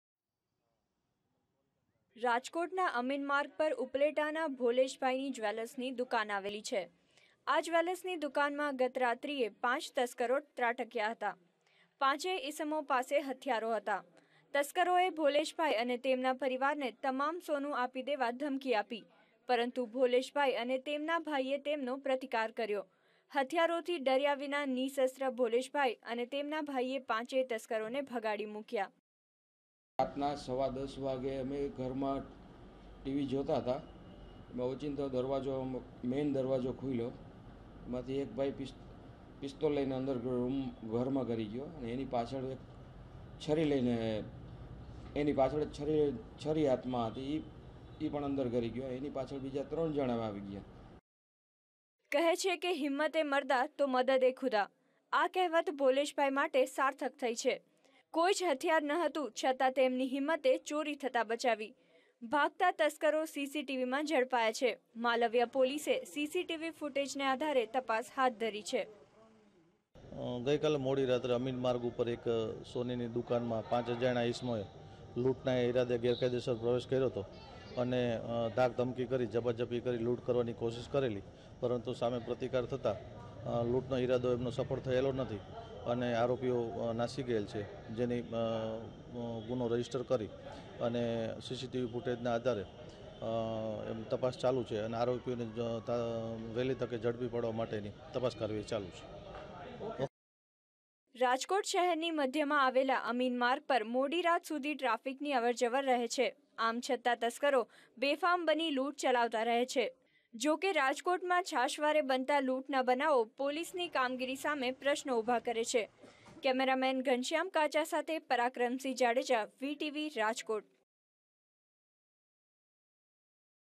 VTV- FAILED ROBBERY IN JERAMBHAI JEWELLS, RAJKOT
રાજકોટના અમીનમાર્ગ પર ઉપલેટાના ભોલેશભાઈની જ્વેલર્સની દુકાન આવેલી છે. આ જ્વેલર્સની દુકાનમાં ગત રાત્રીએ પાંચ તસ્કરો ત્રાટક્યા હતા. પાંચેય ઈસમો પાસે હથિયારો હતા. તસ્કરોએ ભોલેશભાઈ અને તેમના પરિવારને તમામ સોનું આપી દેવા ધમકી આપી. પરંતુ ભોલેશભાઈ અને તેમના ભાઈએ તેમનો પ્રતિકાર કર્યો. હથિયારોથી ડર્યા વિના નિ:શસ્ત્ર ભોલેશભાઈ અને તેમના ભાઈએ પાંચેય તસ્કરોને ભગાડી મૂક્યા.